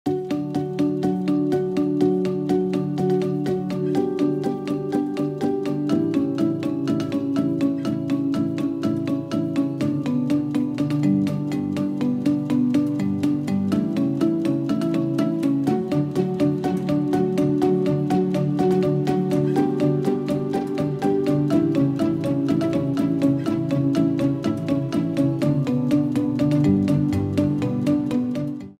Prince William's surprise $33-a-day side hustle
Holidaymakers can enjoy a stay in one of Prince William’s luxury cottages for less than the price of a budget hotel.